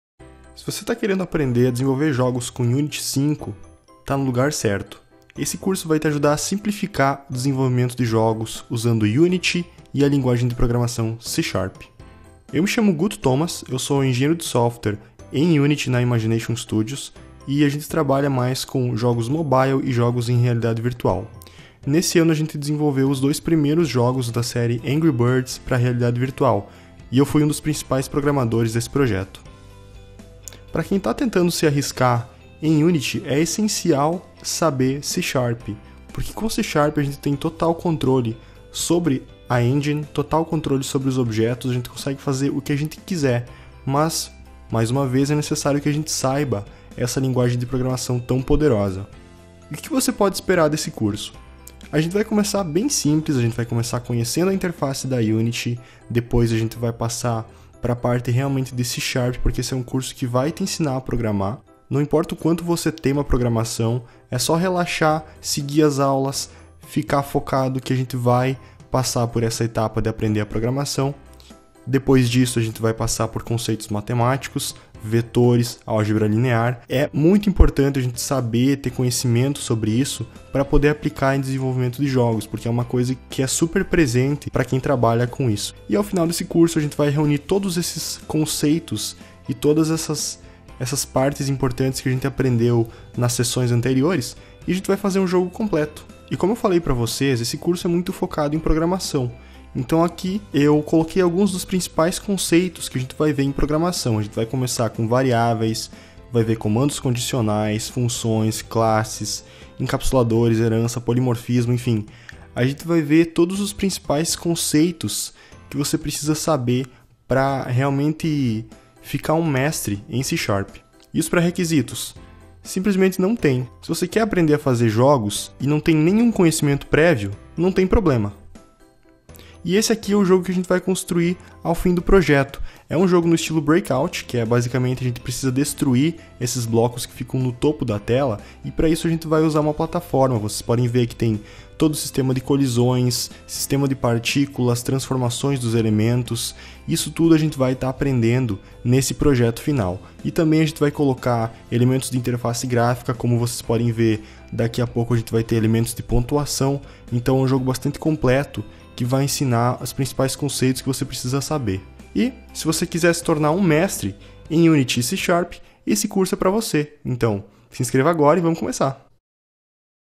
Unity 5 + C# Simplificando o Desenvolvimento de Jogos Udemy
Prepare-se para desenvolver um jogo completo!

Esse curso é indicado para quem está procurando conhecimento de qualidade nos aspectos mais simples de Unity 5 até as funcionalidades mais úteis e complicadas em termos de programação e desenvolvimento de jogos.

Essa não é uma série de vídeo onde eu simplesmente cito os passos para produzir algo. Todos os vídeos são devidamente explicados e a qualidade das aulas pode ser comprovada pelos mais de dez mil alunos que já tive a oportunidade de instruir.

Ao ter acesso a esse material, você verá a programação de um outro modo. Entretanto, não se engane pensando que empenho é opcional. Assim como qualquer outra coisa, aprender a programar não acontece da noite para o dia, então lembre-se de se esforçar em completar testes (incluídos no curso) e seguir as aulas corretamente.

O curso inclui todo o material que será usado nas aulas. Então, tenha certeza de que tudo que você vê em aula é totalmente reproduzível em seu computador.

Esse curso cobre tudo. Desde o mais básico sobre interface até a construção de um game completo! Porém, não espere ter de mergulhar em códigos complicados desde o início. Eu estruturei o curso de tal forma que você aprende tudo sem pressa e no seu ritmo. Nenhum conhecimento prévio é necessário para cada aula a não ser o que você adquiriu na aula anterior.

Quer que eu desenhe? Sem problemas! Para esse curso, desenvolvi vários vídeos conceituais onde explico termos mais complicados com desenhos e diagramas. Uma imagem vale mais que mil palavras, certo?

Posso afirmar que esse é o melhor curso de Unity em português dado o seu preço e a qualidade do material oferecido. Espere um instrutor dedicado com grande experiência prévia (meu curso de Unity em inglês é um sucesso. Pense nesse como uma versão melhorada!). Confie em mim quando eu digo: Eu falo a sua lingua!

Podemos começar?